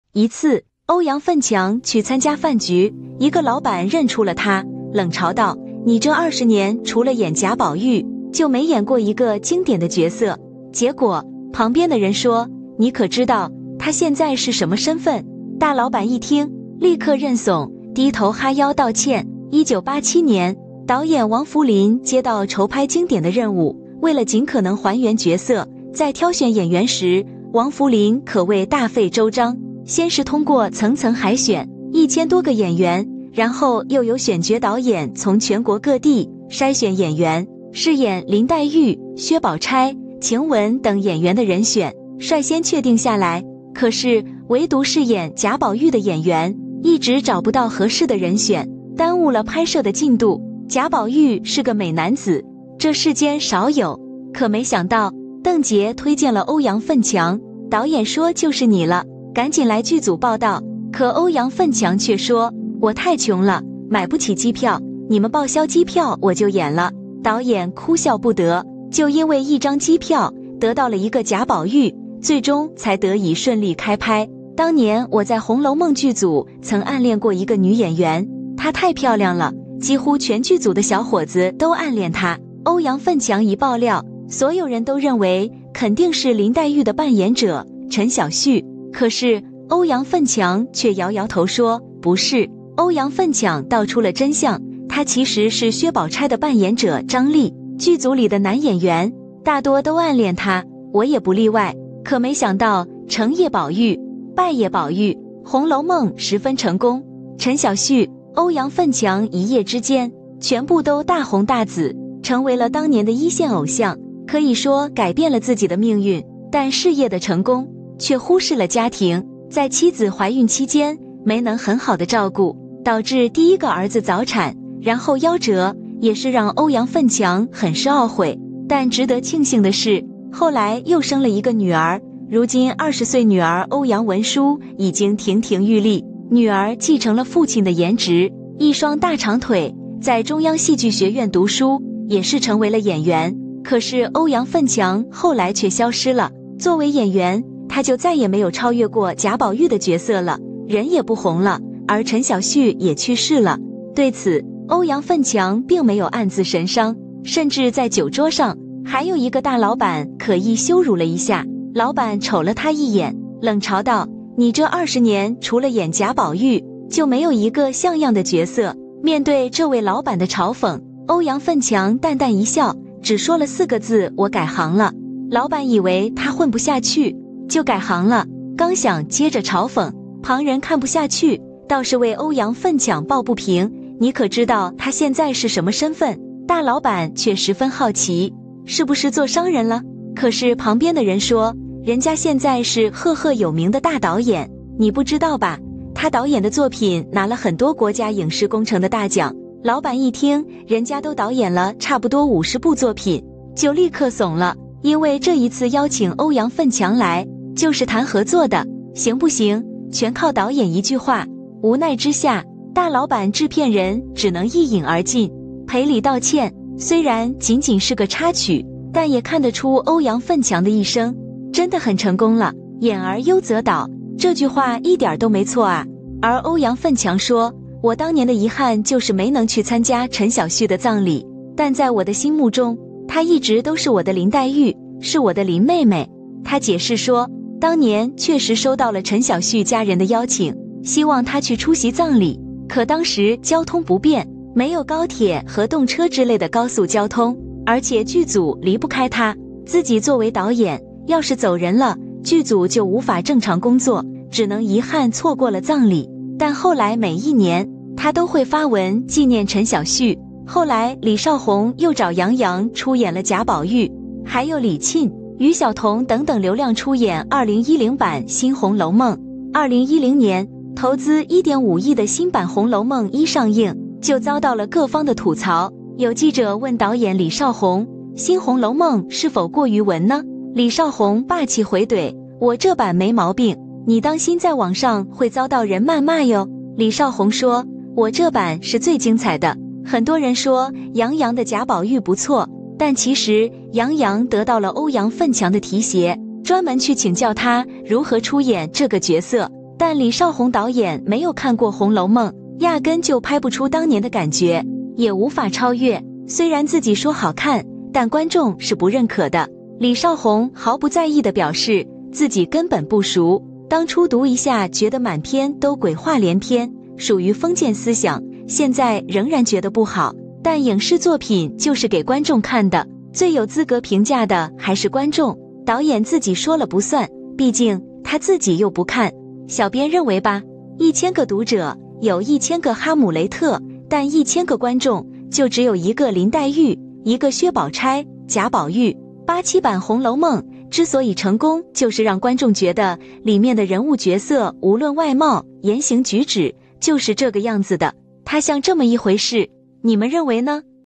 歐陽奮強曾被人嘲道：20年就了演賈寶玉！可知道他身份後立刻道
欧阳奋强曾被人嘲道：20年就了演贾宝玉！可知道他身份后立刻道